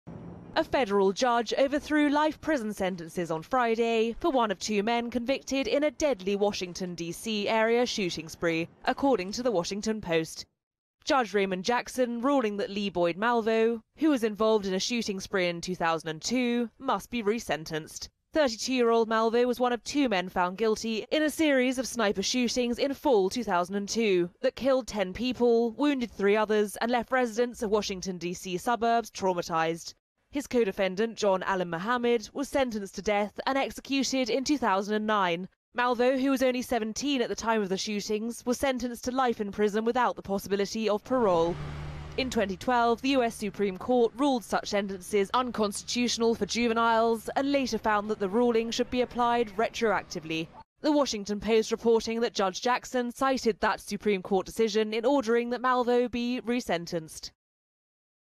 Judge overturns life sentences for DC sniper Lee Boyd Malvo - Daily Mail
The notorious Beltway sniper Lee Boyd Malvo had four life sentences thrown out by a federal judge on Friday because they were retroactively deemed unconstitutional. In his ruling, US District Judge Raymond Jackson in Norfolk, Virginia, said Malvo is entitled to new sentencing hearings after the US Supreme Court ruled mandatory life sentences for juveniles are unconstitutional. Malvo was 17 when he was arrested in 2002 for a series of shootings that killed at least 10 people and wounded three others in Virginia, Maryland and the District of Columbia, causing widespread fear throughout the region.